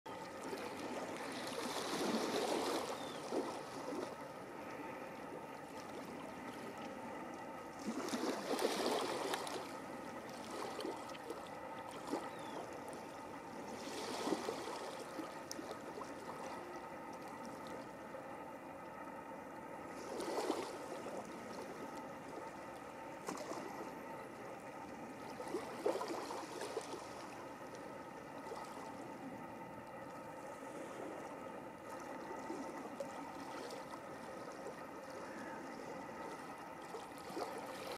Craw enjoying seafood.
A carrion craw(Corvus corone) eating a small crab.